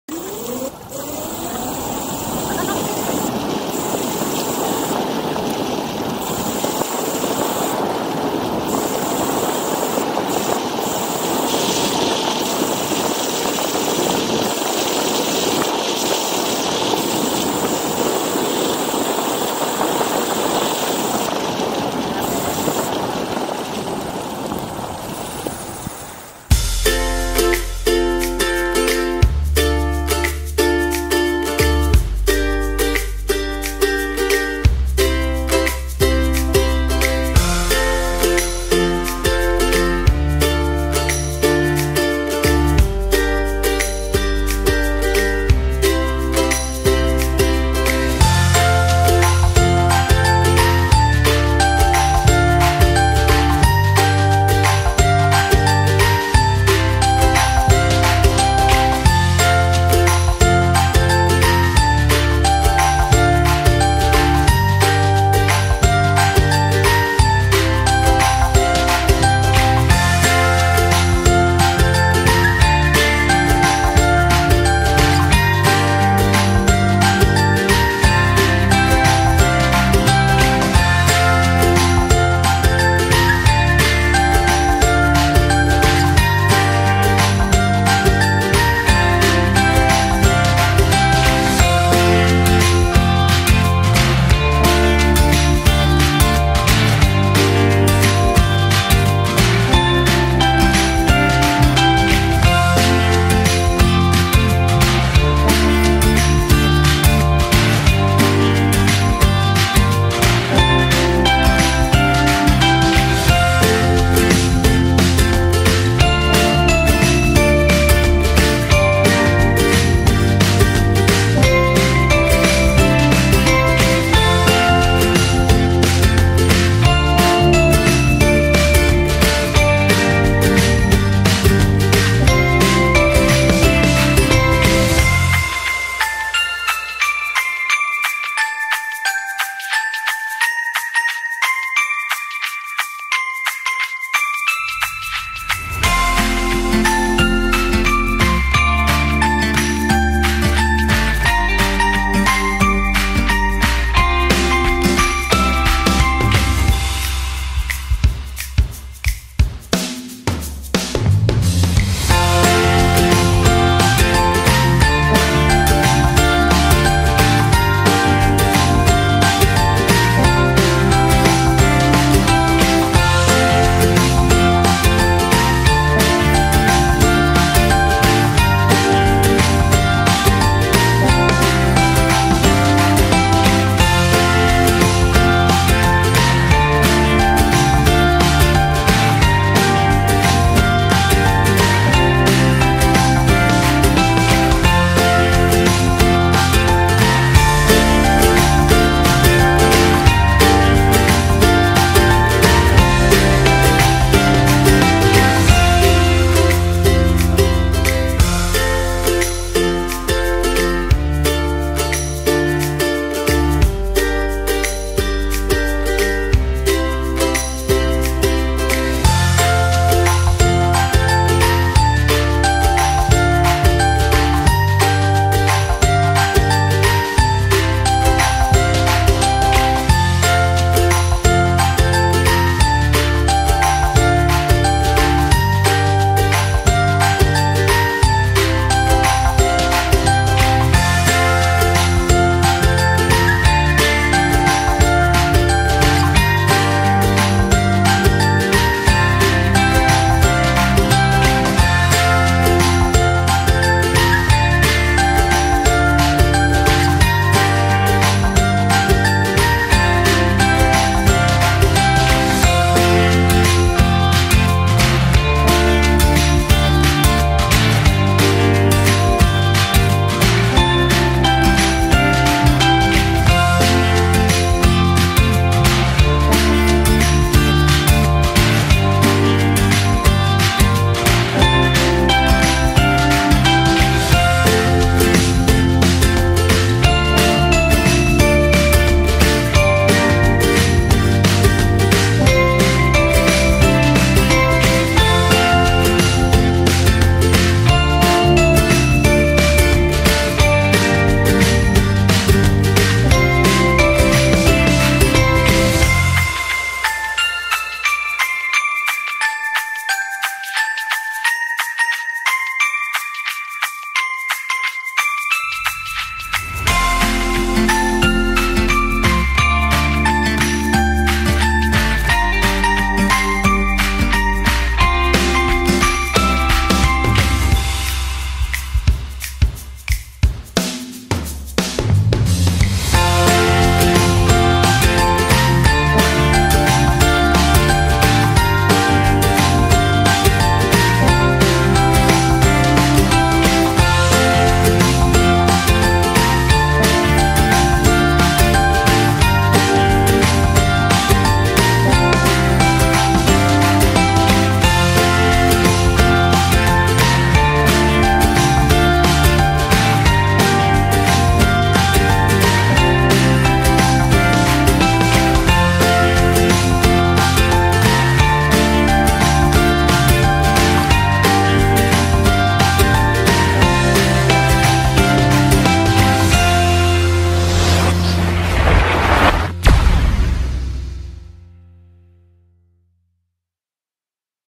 Cavite Loop with Kaabo Wolf Warrior
190kms Solo Ride to explore Cavite province with Kaabo Scooter